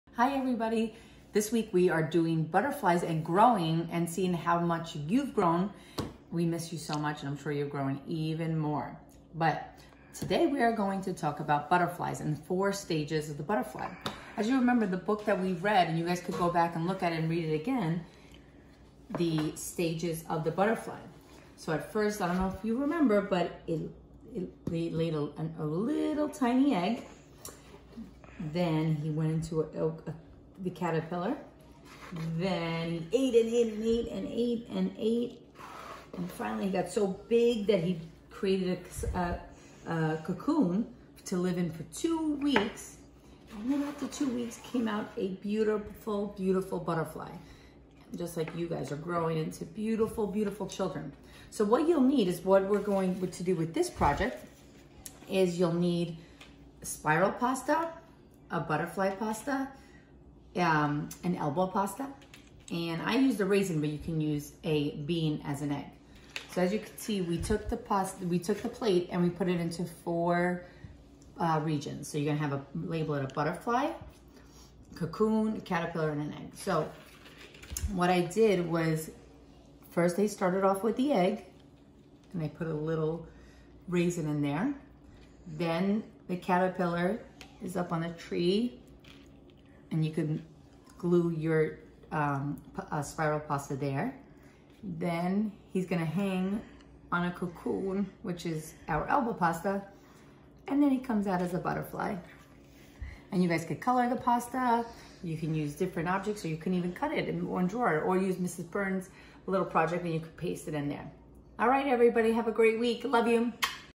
Create the Lifecycle of a butterfly with pasta
What you will need:
Bean or a raisin (egg)
Spiral pasta (caterpillar)
Elbow or ear pasta (cocoon)
Butterfly pasta